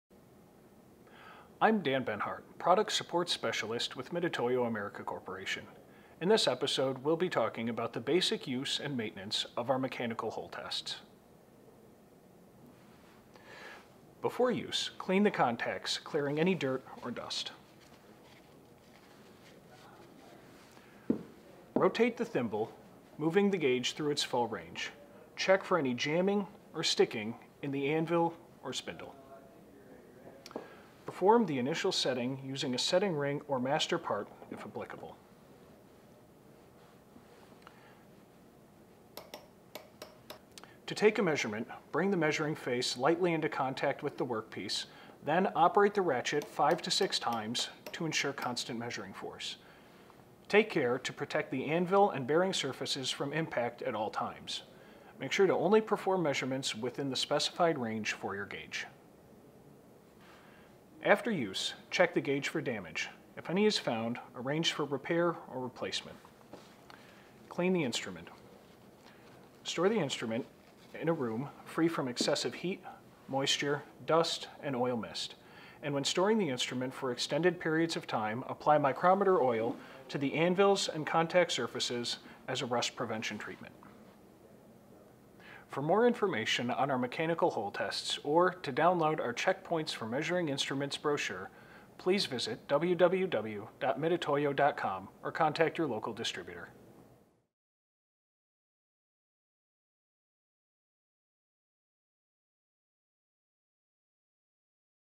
Check Points for Measuring Instruments - Vernier Holtest
Check Points for Measuring Instruments - This video covers the proper use of Vernier Holtests from Mitutoyo.